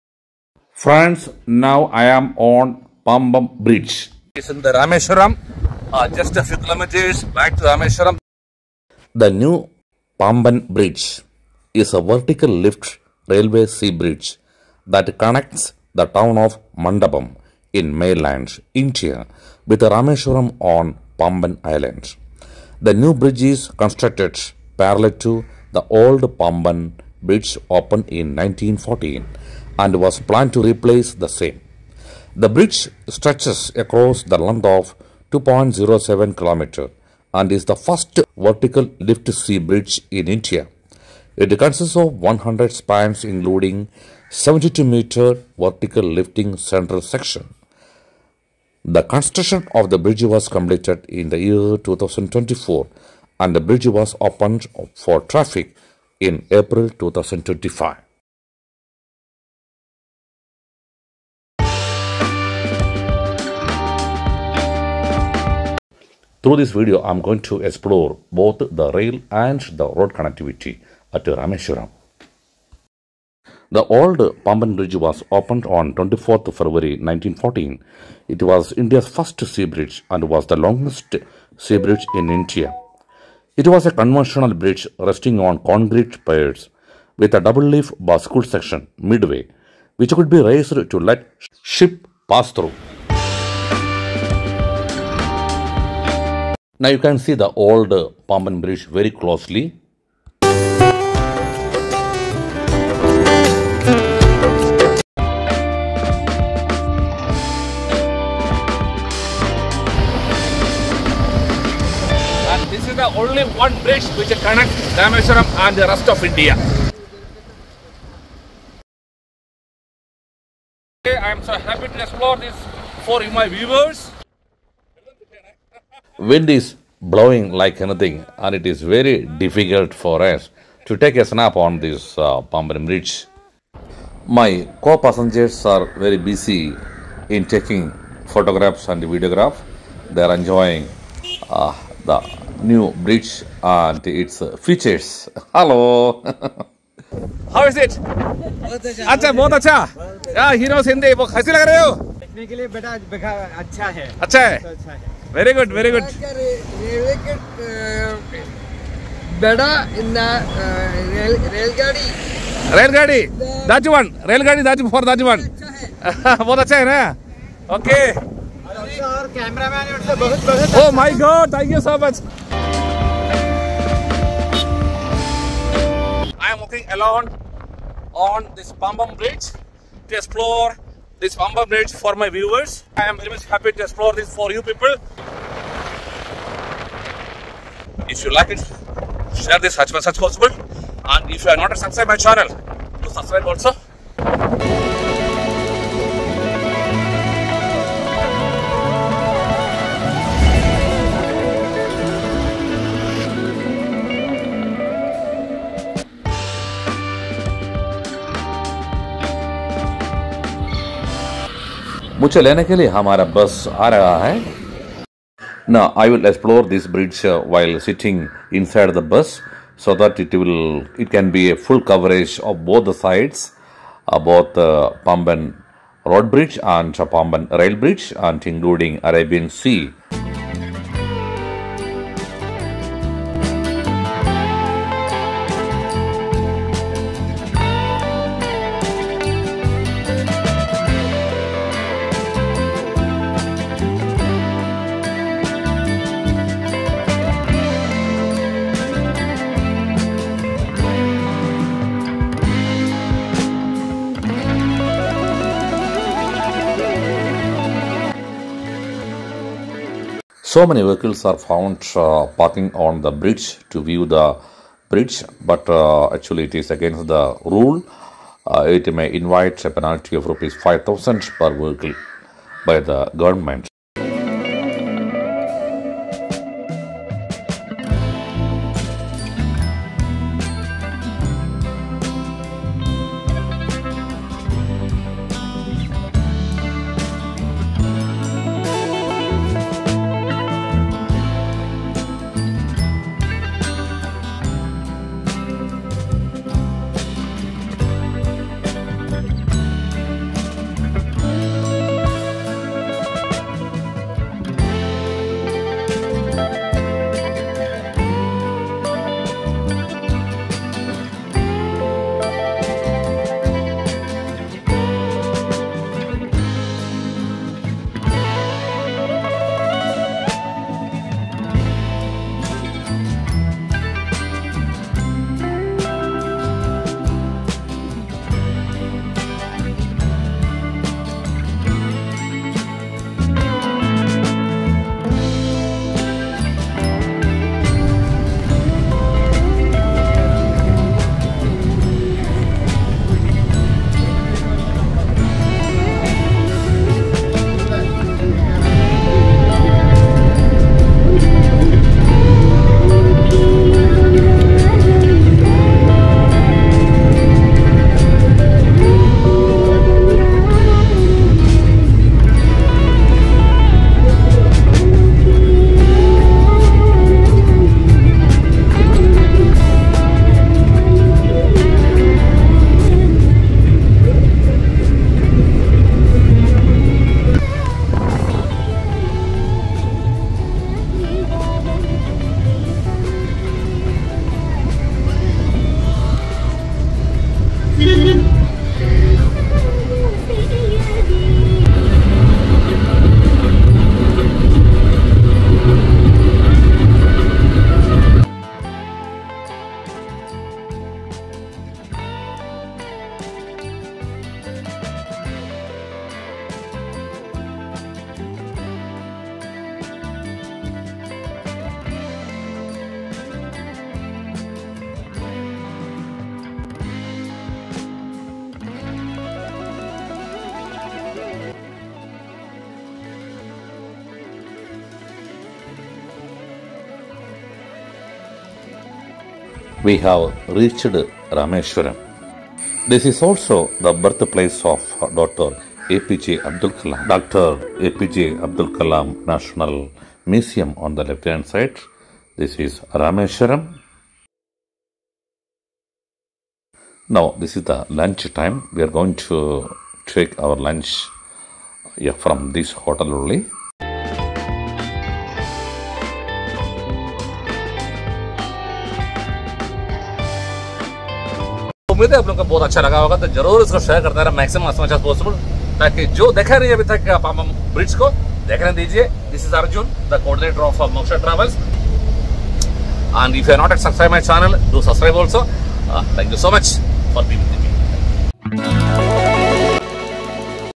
New Pamban Bridge : India's First Vertical- Lift Railway Sea Bridge
Join me as I walk along the historic Pamban Bridge in Rameswaram, Tamil Nadu—the first sea bridge in India and an engineering marvel. Watch the magic as the bridge opens for ships and witness the scenic Bay of Bengal like never before!